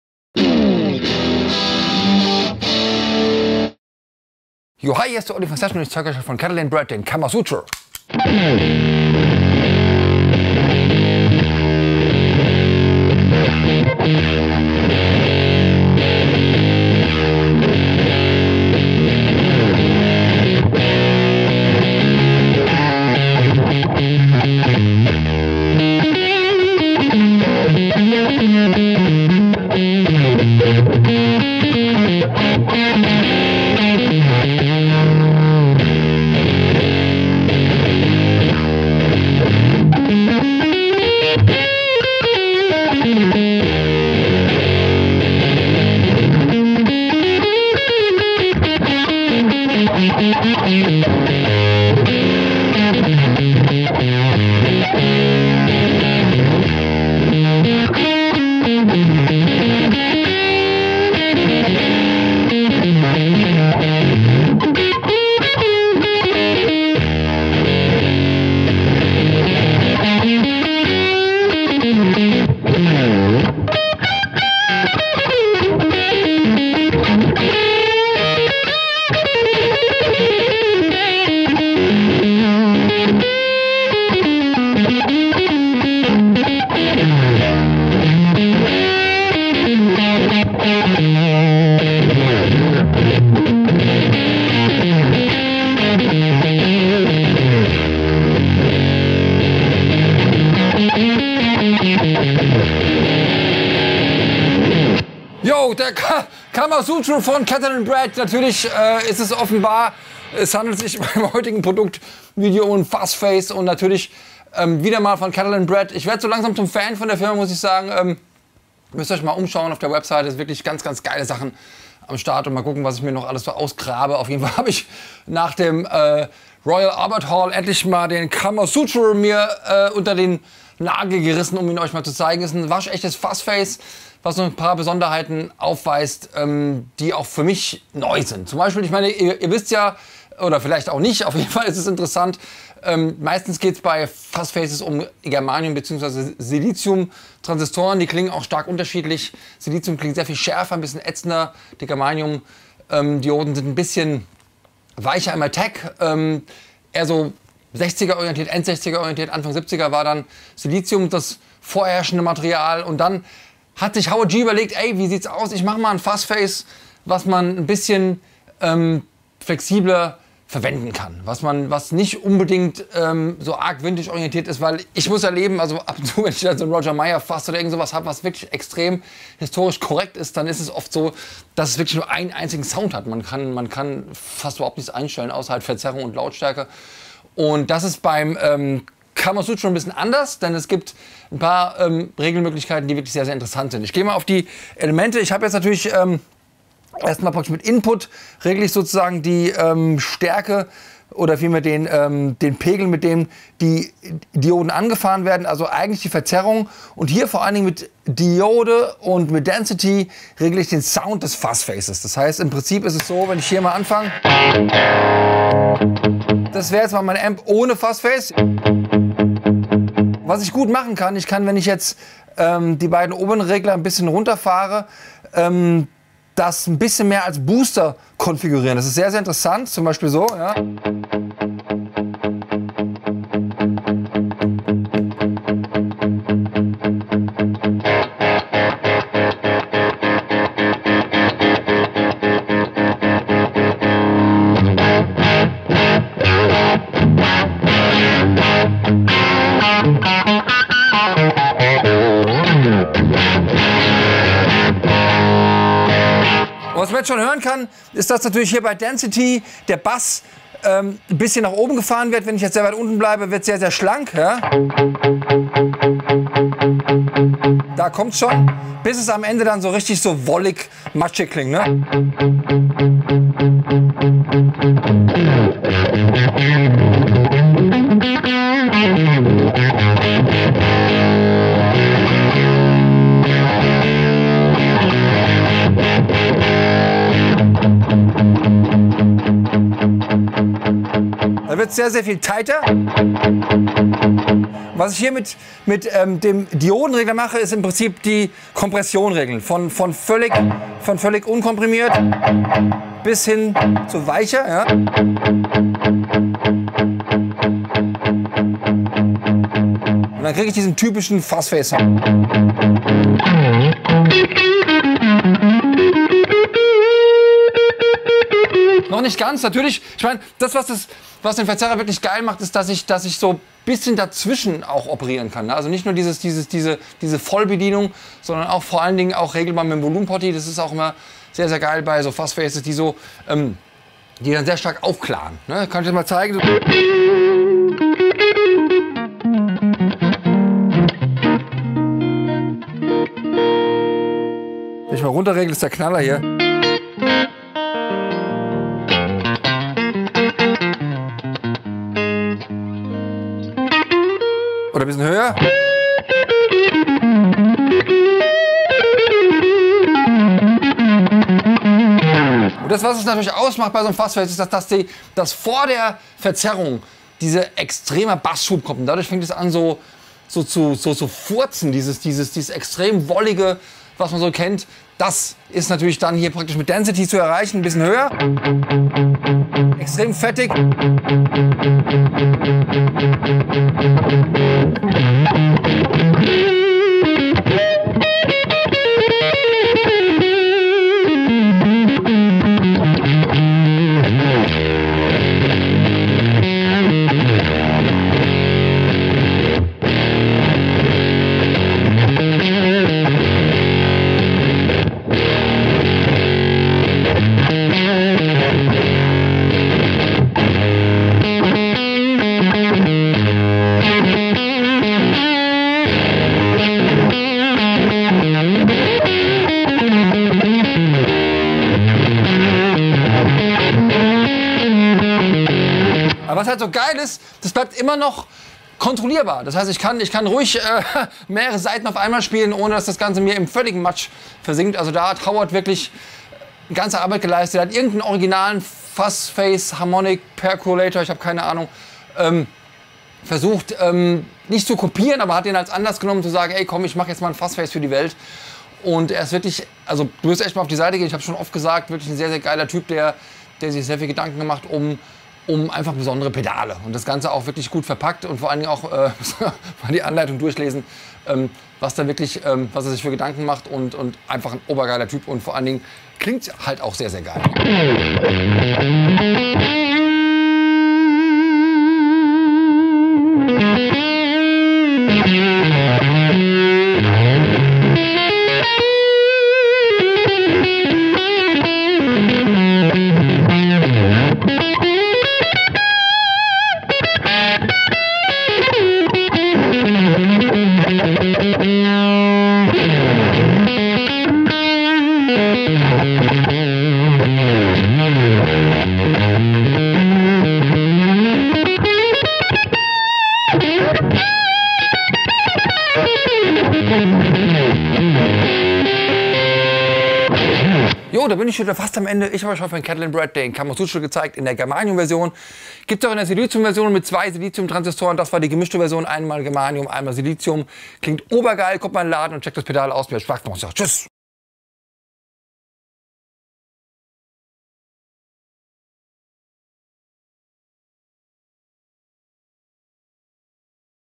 Catalinbread Karma Suture Overdrive (im Test)
Mit dem Catalinbread Karma Suture OD Pedal zeigt euch Olli im Video quasi ein Fuzz Face, das aber noch ein paar zusätzliche Features aufweist. Eine weitere Besonderheit ist die Kombination aus Germanium- und Silicium-Transistoren, die für den dynamischen, warmen Klang verantwortlich ist.